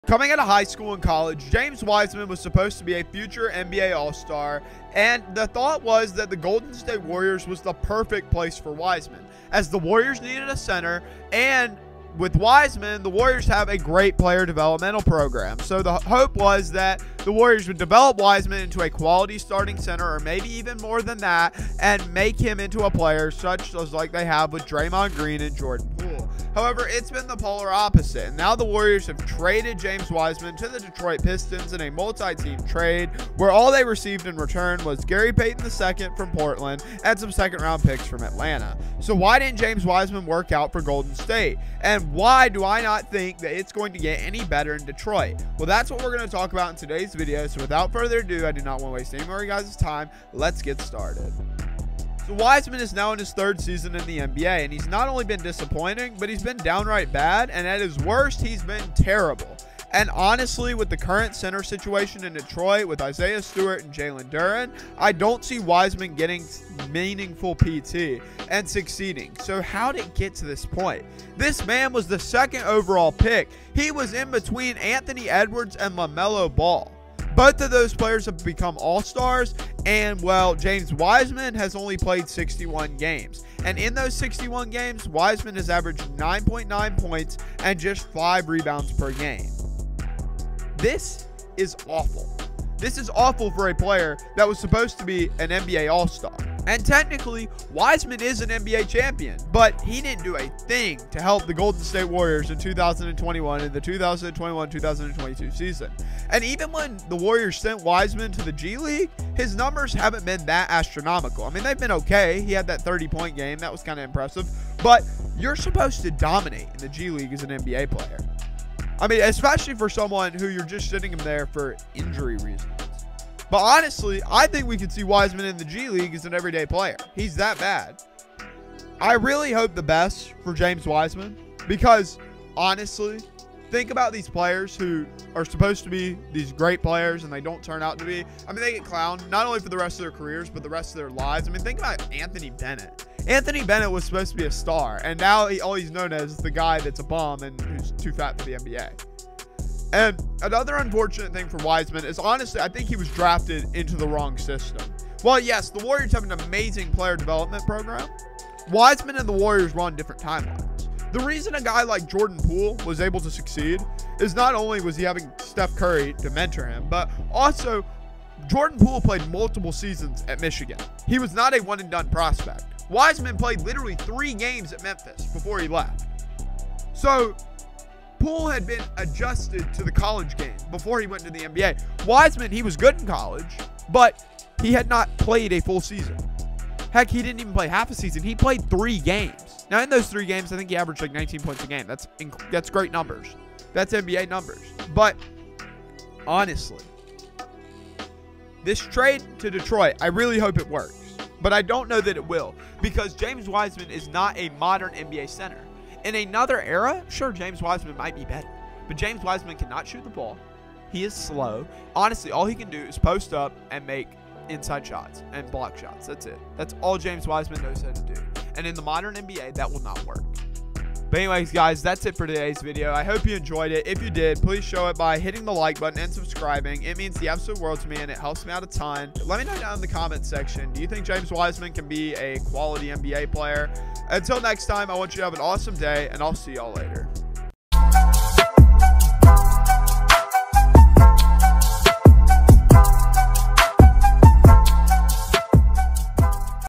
James Wiseman is Terrible
James Wiseman has been the polar opposite of what many thought he would he’s been a huge let down today we talk about the future of his NBA career

By waytoolost